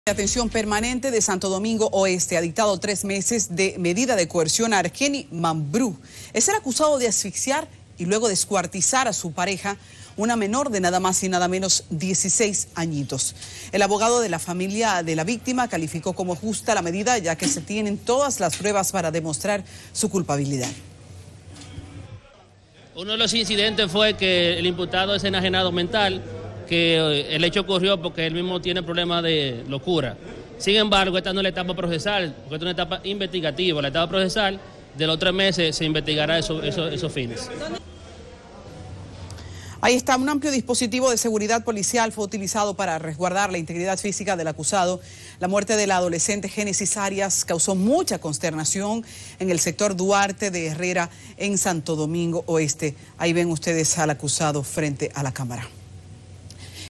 Dictan tres meses de prisión preventiva a hombre mato y descuartizo a su pareja en Herrera
La Oficina de Atención Permanente de Santo Domingo Oeste dictó 3 meses medida de coerción a Argeny Mambru acusado de asfixiar y luego descuartizar a su pareja, una menor de 16 años.

El abogado de la familia de la víctima, calificó como justa la medida ya que se tienen todas las pruebas para demostrar su culpabilidad.